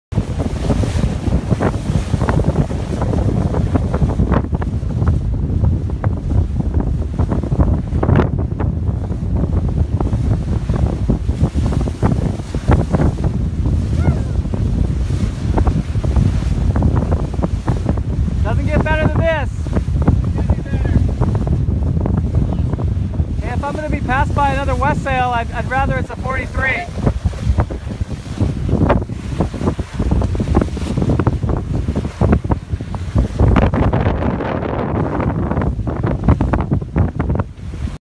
2018 Westsail Parade - Niniwahuni W43 passed Tortuga W32
Ted Crocker's Niniwahuni Westsail 43 and Randy Leasure's Tortuga Westsai 32 sail close together. Video by Randy Leasure.